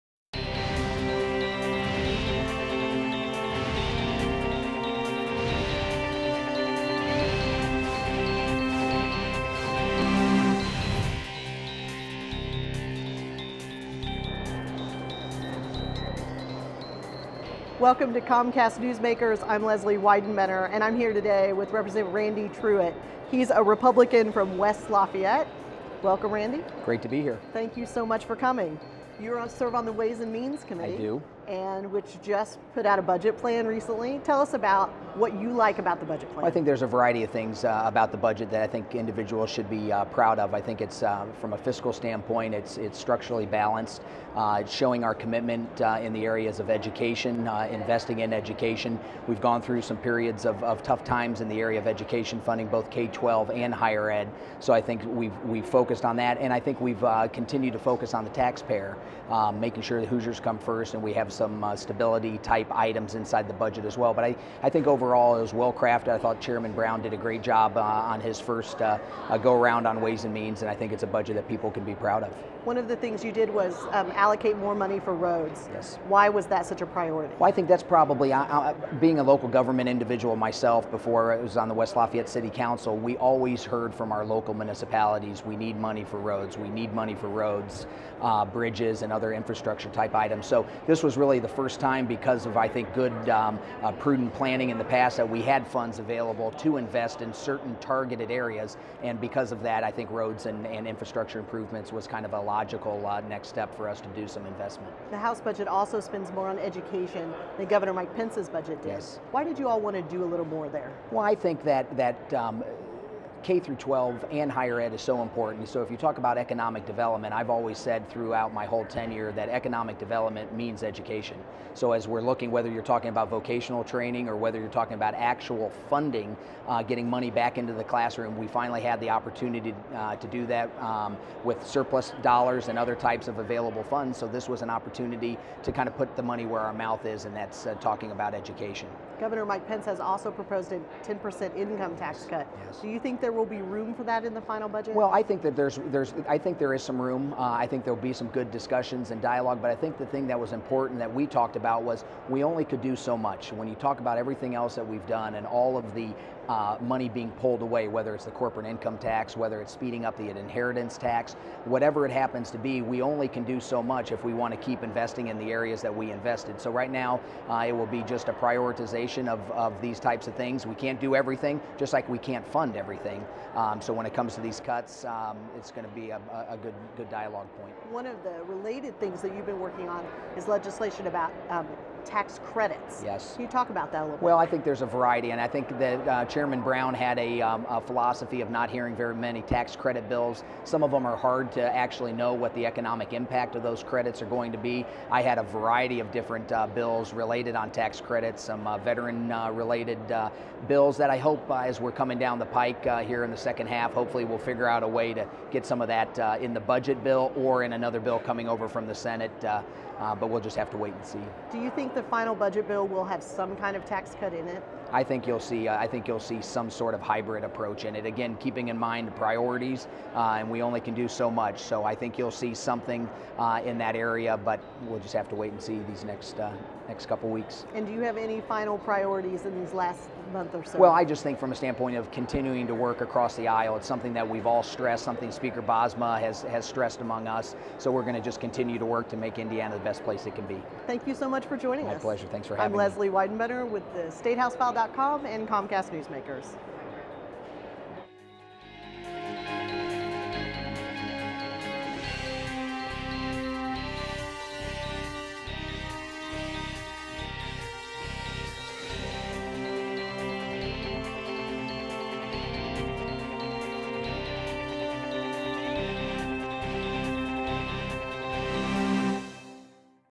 Prioritizing Cuts-Rep. Randy Truitt 2013
(R) West Lafayette.  Representative Randy Truitt discusses the reasons for the House budget spending more on education than Gov. Pence's budget does.